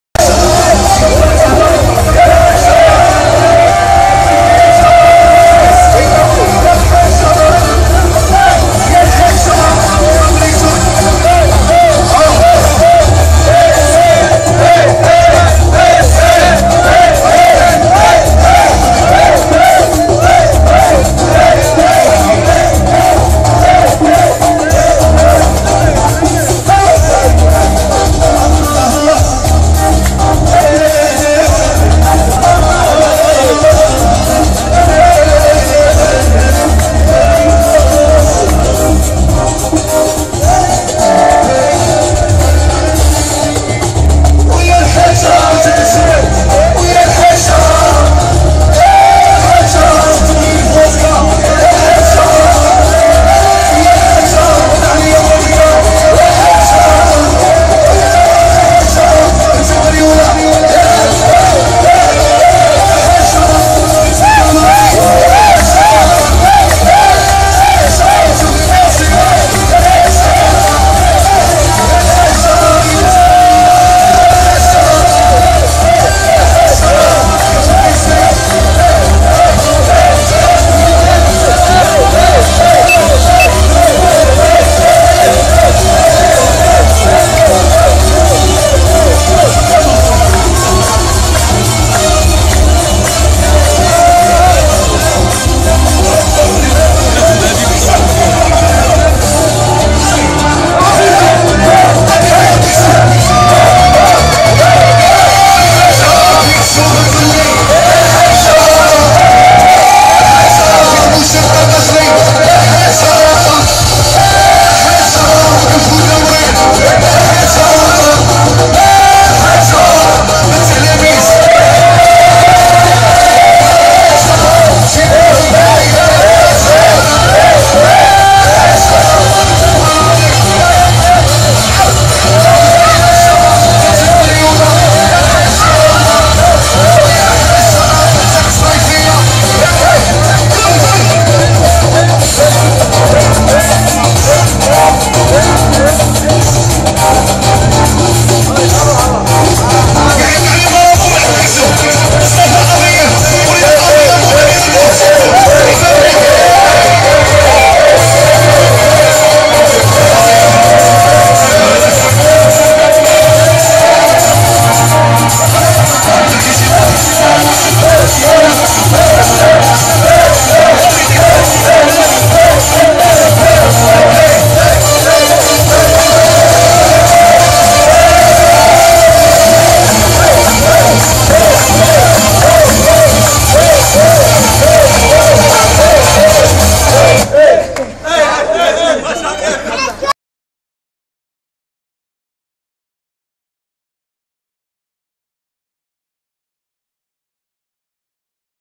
soiréé de mon fiancaille pffff hallaa vc alpella.rabie hallla dernaha kahlou kahllouuu
je dit mrc 3likoum hbabi ziniin w khoutii w jma3tii zina limatih matwelii march ariérre ma3adnach mout 3likoum khoutti hamouda réda sadouk mon frér+haraka chriki mon bra droit intim et bachir spanki hbibi w tt amin 100 hachmi lavage khouyya hh et néztoz amine  bounia hbibi hbib khoua malliaaaaaaaard w l3awnii hata lel sbah hhh mout 3likoum khouttii wlh illaaa haylloullaaa fooooooooorttt